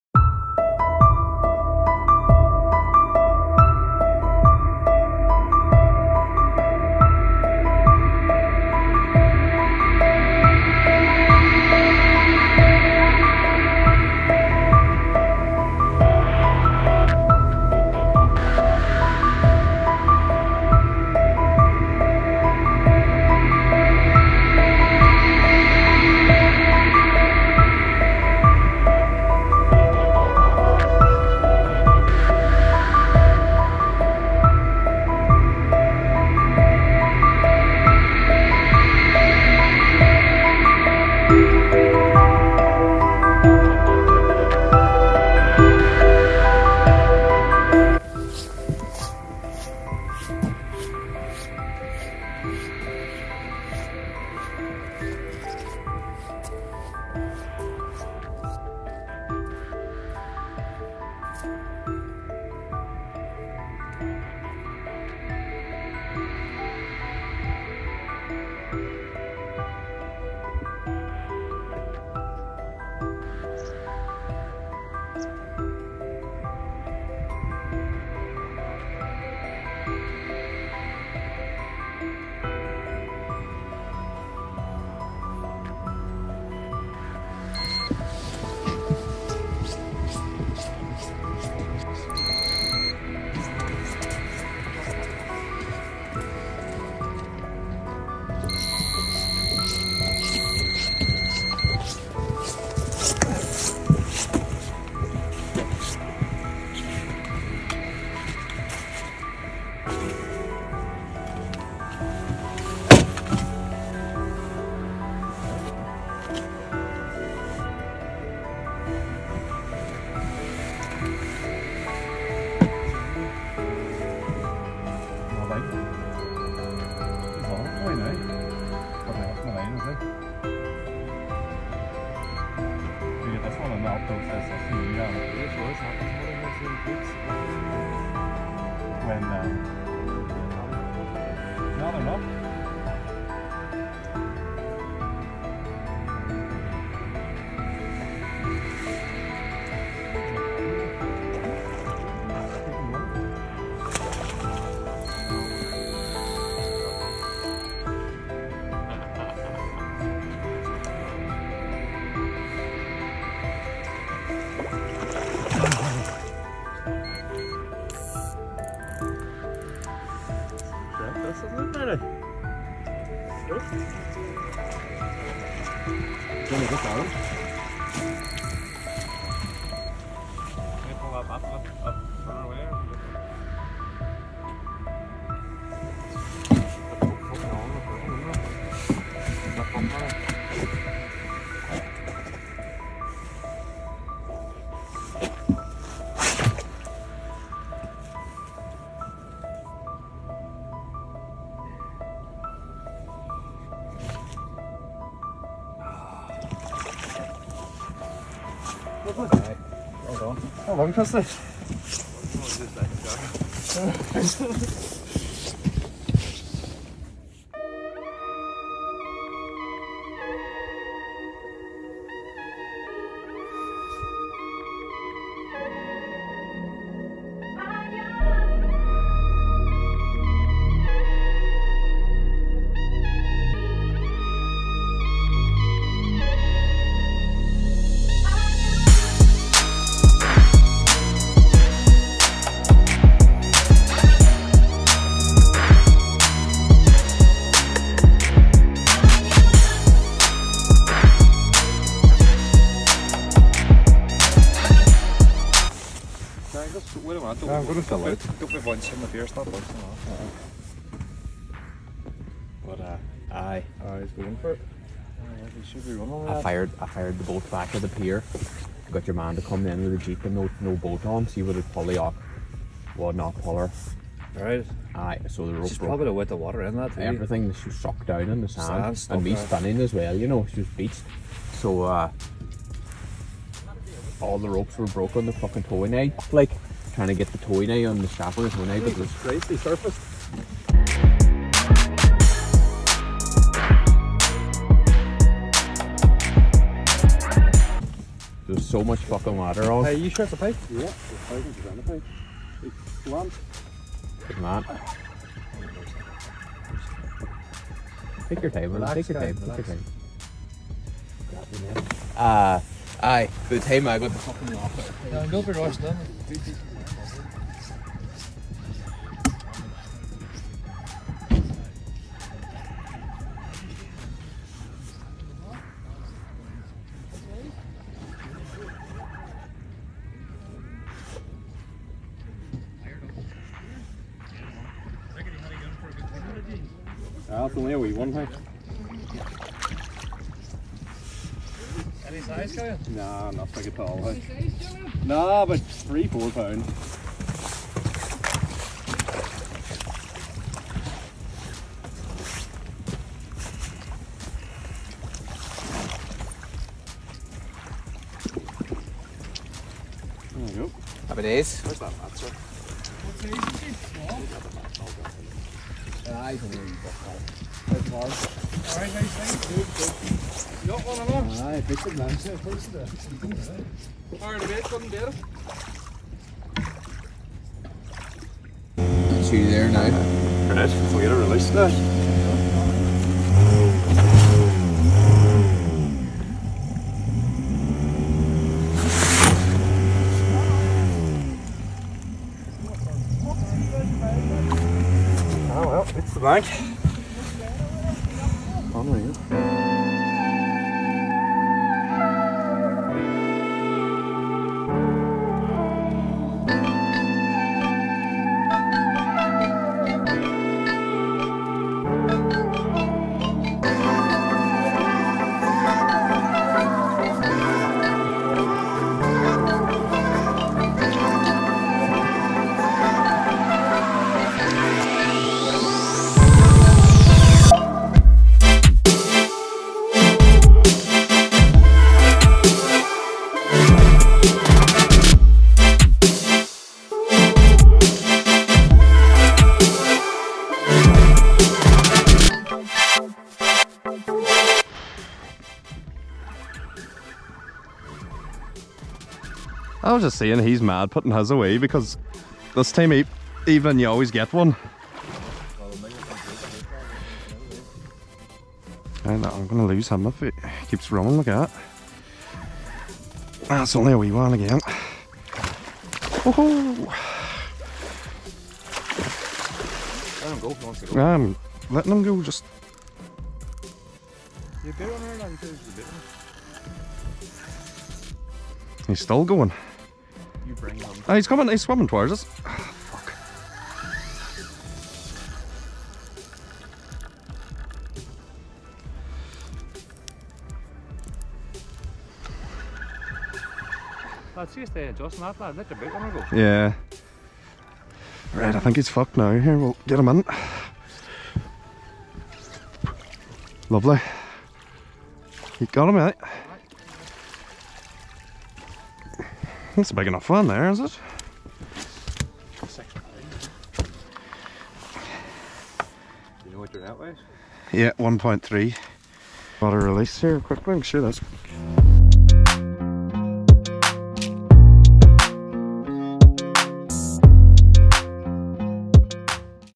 Lough Erne Pike Fishing!
Fishing for Pike on the River Erne, Co Fermanagh, Ireland.